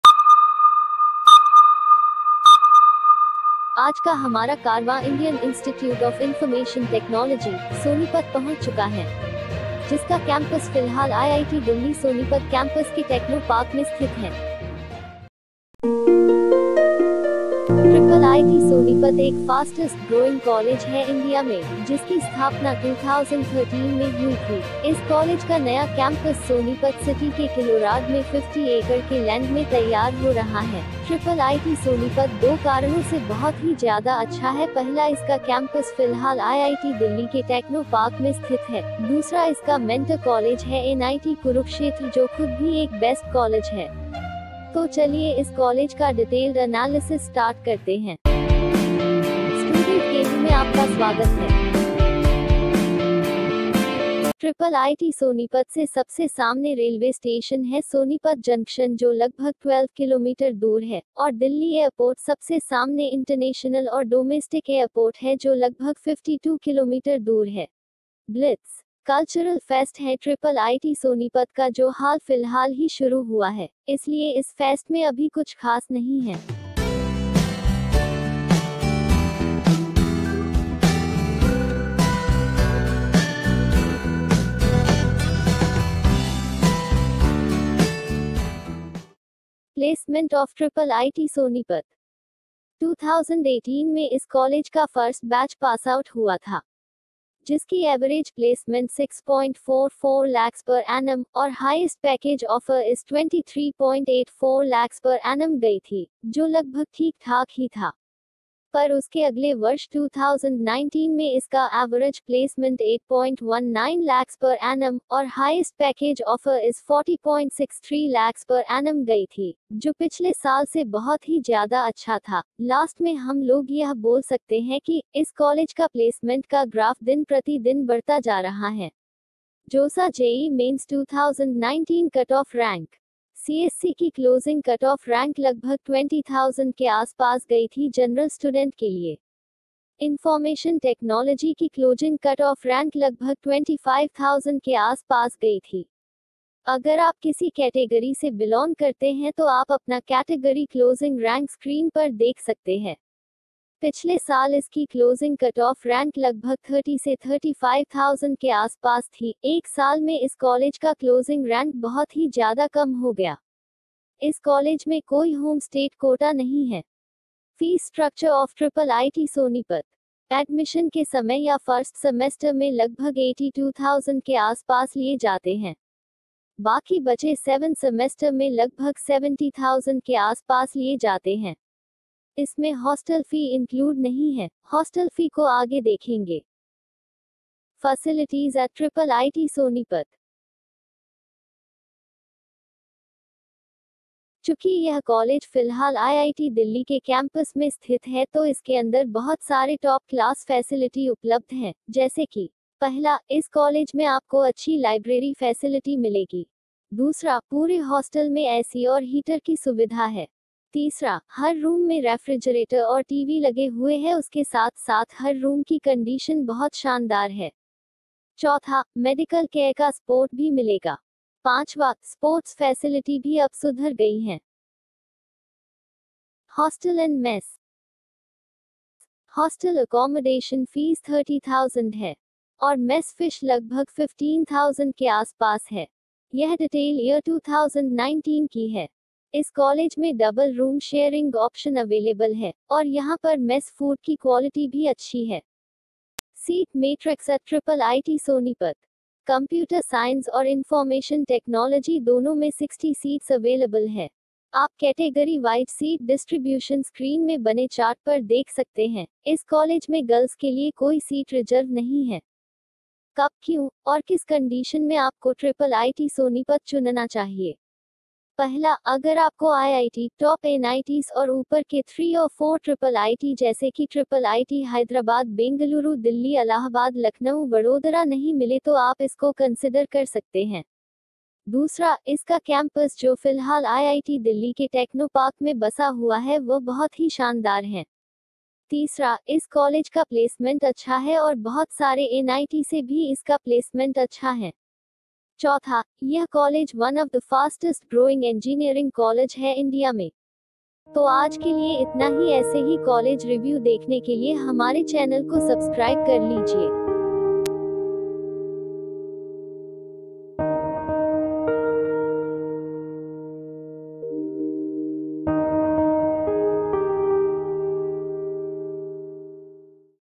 IIIT SONEPAT | Campus Tour | Placement | Facilities | Jee Mains Cuttoff | Fees Structure | Fests..
Huy guys,
This college review is on IIT Sonepat.IIIT Sonepat has a temporary campus which is at IIT DELHI Techno Park Sonepat. The campus was just osam with all luxury facilities.

                                                            This college is one of the fastest growing engineering college in India.

IIIT Sonepat Placement Statistics is also a advantage of this college.

iiitsonepat.ac.in
  IIIT Sonepat

For more interesting college reviews